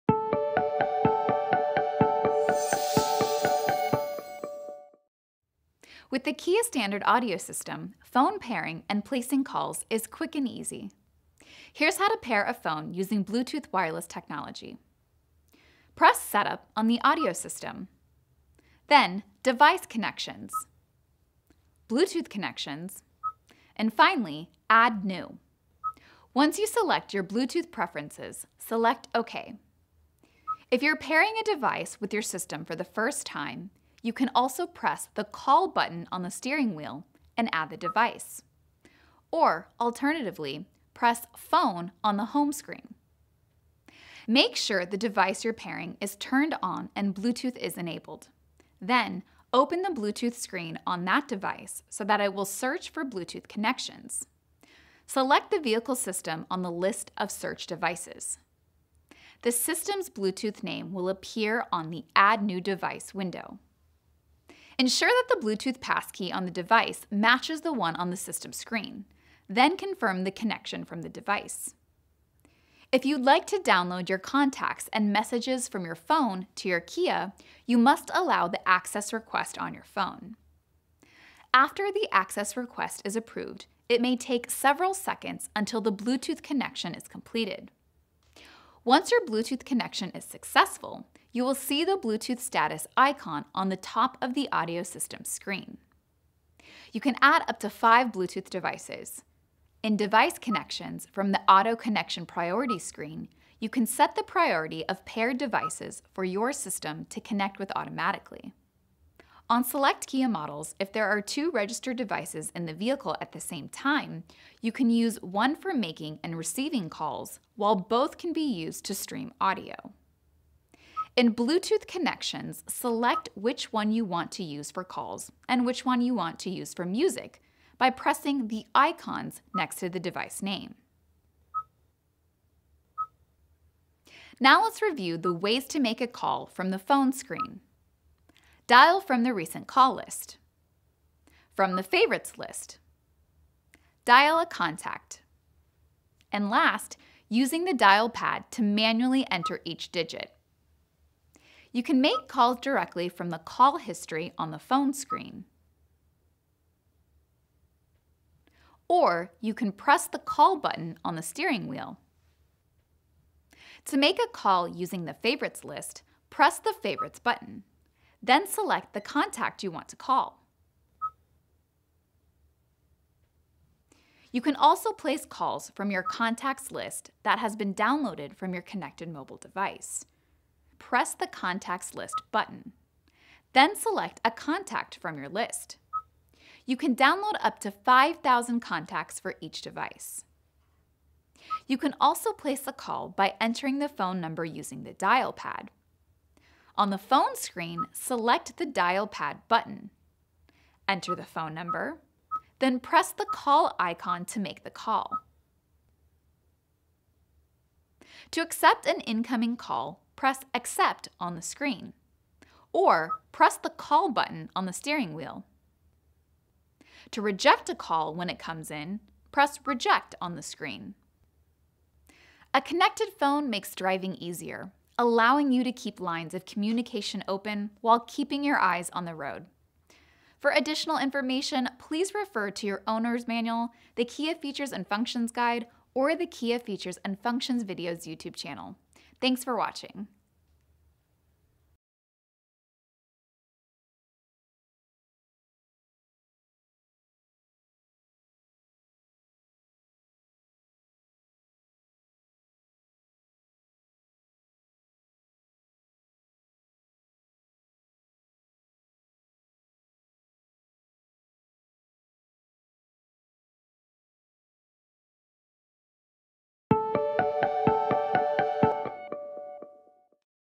PHONE OPERATION: Pairing and Making Calls
With the Kia Standard Audio System, phone pairing, and placing calls is quick and easy …
Here’s how to pair a phone using Bluetooth® Wireless Technology:
Press SETUP on the audio system …
Then DEVICE CONNECTIONS …
BLUETOOTH CONNECTIONS …
And finally, ADD NEW.
Once you select your Bluetooth preferences, select OK.
If you’re pairing a device with your system for the first time, you can also press the CALL button on the steering wheel and add the device…
Or, alternatively, press PHONE on the HOME screen.
Make sure the device you’re pairing is turned on and Bluetooth is enabled – then open the Bluetooth screen on that device so that it will search for Bluetooth connections.

Select the vehicle system on the list of searched devices.
The system’s Bluetooth name will appear on the “ADD NEW DEVICE” window.
Ensure that the Bluetooth Passkey on the device matches the one on the system screen – then confirm the connection from the device.
If you’d like to download your contacts and messages from your phone to your Kia, you must allow the access request on your phone.
After the access request is approved, it may take several seconds until the Bluetooth connection is completed.
Once your Bluetooth connection is successful, you will see the Bluetooth status icon at the top of the audio system screen.
You can add up to five Bluetooth devices.
In DEVICE CONNECTIONS, from the AUTO CONNECTION PRIORITY screen, you can set the priority of paired devices for your system to connect with automatically.
On select Kia models, if there are two registered devices in the vehicle at the same time, you can use one for making and receiving calls, while both can be used to stream audio.
In BLUETOOTH CONNECTIONS, select which one you want to use for calls and which one you want to use for music by pressing the ICONS next to the device name.

Now let’s review the ways to make a call from the PHONE screen:
Dial from the Recent Call list ...
From the Favorites list ...
Dial a contact …
And last, using the dial pad to manually enter each digit.
You can make calls directly from the Call History on the PHONE screen…
Or you can press the CALL button on the steering wheel …
You will see RECENT CALLS …
Then select the number you want to call.
To make a call using the Favorites List, press the FAVORITES button…
then select the contact you want to call.
You can also place calls from your CONTACTS LIST that has been downloaded from your connected mobile device.
From the PHONE screen, press the CONTACTS LIST button …
Then select a contact from the list.
You can download up to 5,000 contacts for each device.
You can also place a call by entering the phone number using the dial pad.
On the PHONE screen, select the DIAL PAD button …
Enter the phone number …
Then press the CALL icon to make the call.
To accept an incoming call, press ACCEPT on the screen…
Or press the CALL button on the steering wheel.
To reject a call when it comes in, press REJECT on the screen…
A connected phone makes driving easier, allowing you to keep lines of communication open while keeping your eyes on the road.